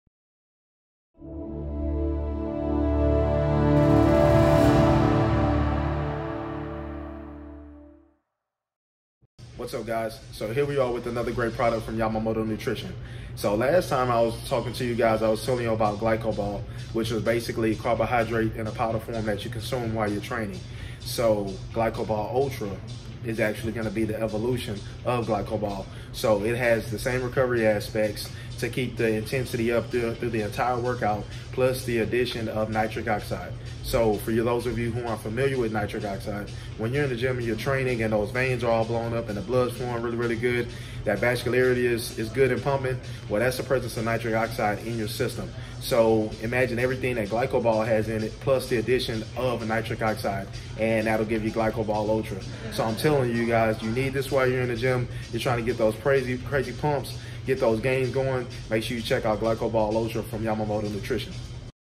GlycoBol® ULTRA | The Carbs evolution for your Workout - By Patrick Moore
GlycoBol® ULTRA is an innovative formula for anyone who is looking for an uncompromising performance product, energy, and stimulation of nitric oxide (ENO-1 ™ technology). Discover more